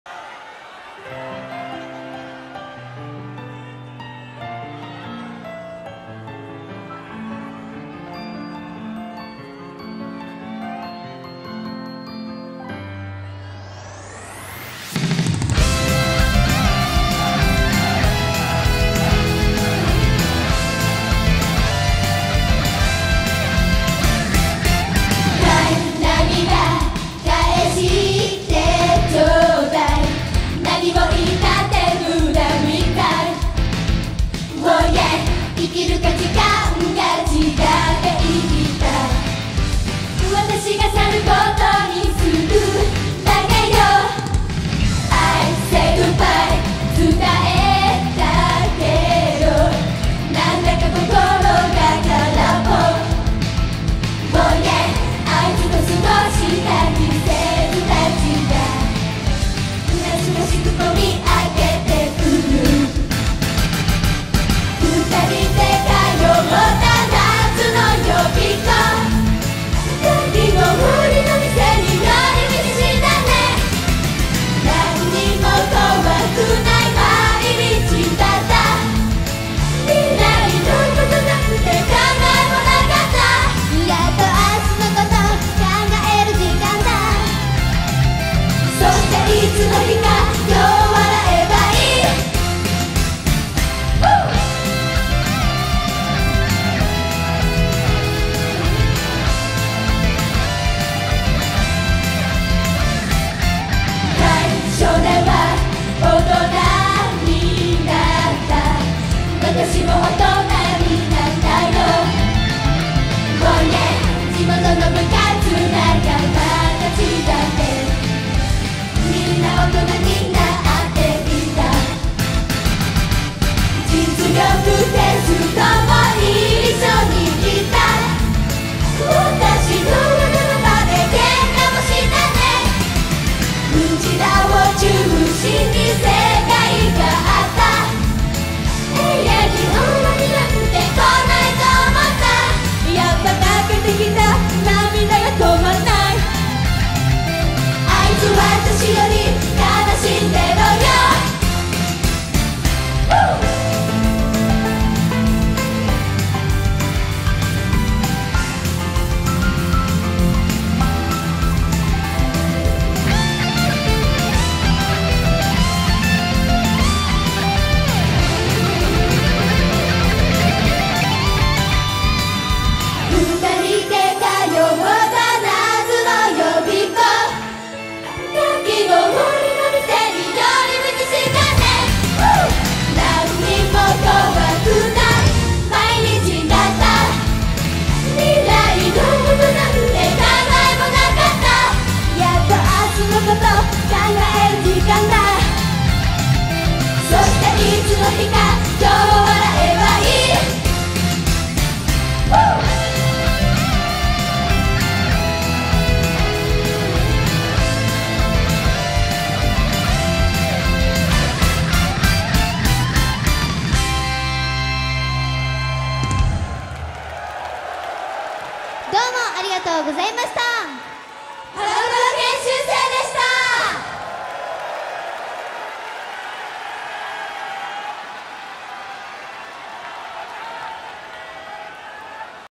Crying ハロプロ研修生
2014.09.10
℃-ute / Berryz工房「℃-ute（910）の日スペシャルコンサート2014 Thank you ベリキュー！in 日本武道館［前編］

田辺奈菜美(ex.OnePixcel)
浜浦彩乃(ex.こぶしファクトリー)
田口夏実(ex.こぶしファクトリー)
小川麗奈(ex.こぶしファクトリー)
室田瑞希(ex.アンジュルム)
山岸理子(つばきファクトリー)
野村みな美(ex.こぶしファクトリー)
一岡伶奈(BEYOOOOONDS / CHICA # TETSU)
加賀楓(ex. モーニング娘。'22)
佐々木莉佳子(アンジュルム)
羽賀朱音(モーニング娘。'23)
相川茉穂(ex.アンジュルム)
斎藤夏奈